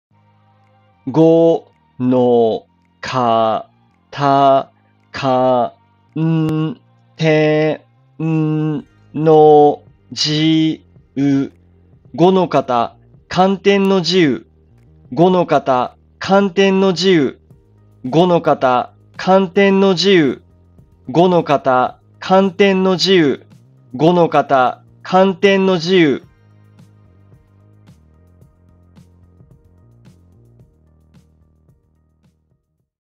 Tanjiro's water breathing 5th form technique "Blessed rain after drought" in Japanese Pronunciation
This is how you pronounce Tanjiro Kamado's technique Kanten no Jiu from Demon Slayer in Japanese. In this channel, I, a native Japanese speaker, will introduce a variety of Japanese words and phrases with a focus on pronunciation.